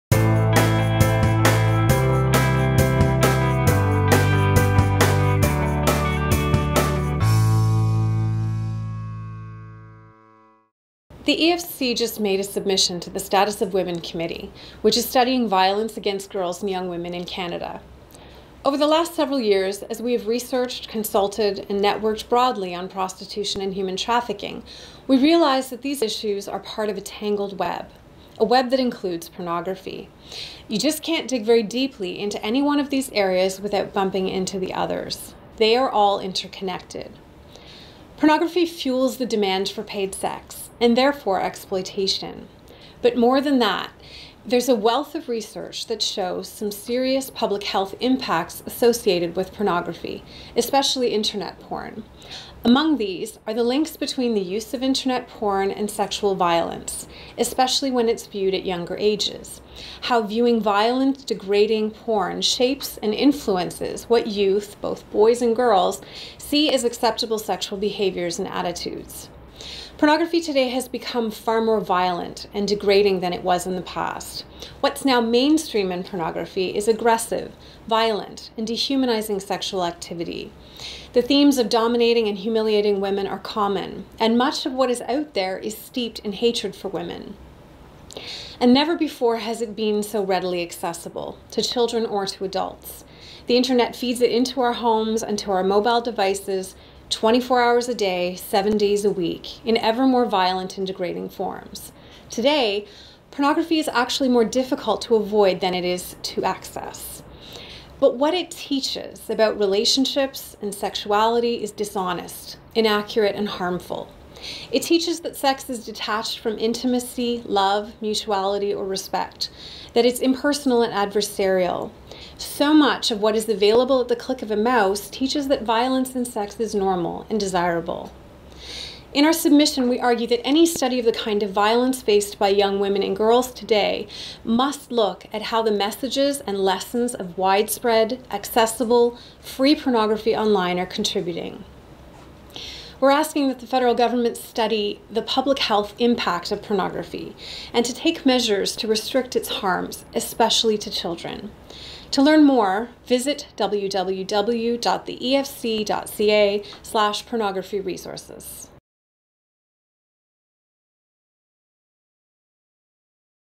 Links between violent online pornography and violence against girls and young women
EFC Director of Public Policy highlights the EFC's submission to the Status of Women Committee on the links between consumption of violent online pornography and violence against girls and young women.